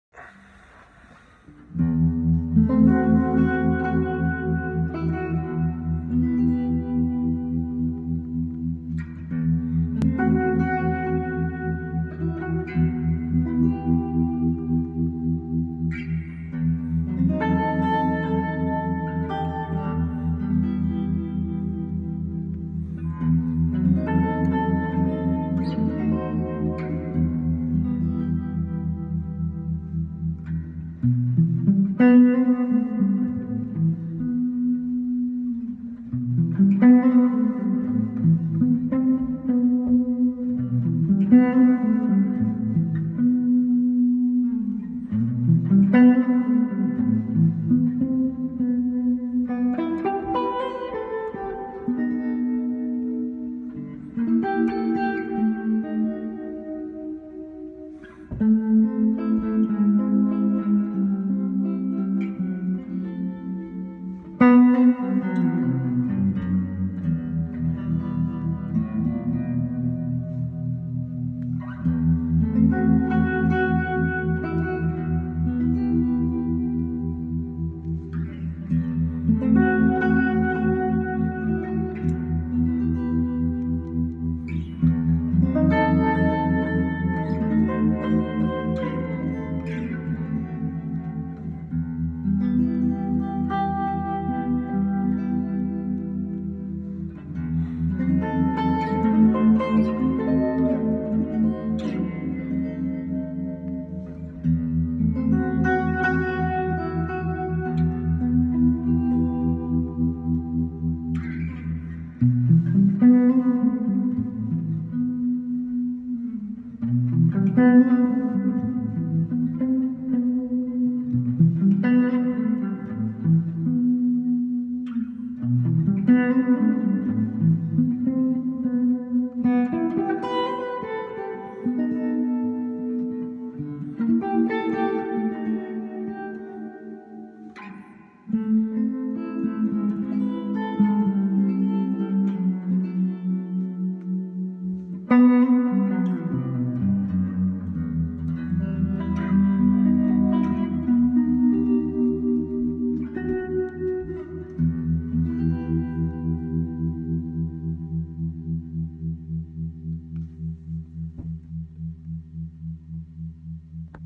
Strymon Flint inspired noodling....
Just got myself an early XMAS present..the Strymon Flint pedal. Never played a pedal before that immediately inspires and creates a mood. Love this thing!

Gear
52 Fender Custom Shop Telecaster
Princeton Reverb
Strymon Flint (1st Wave form / 70s reverb)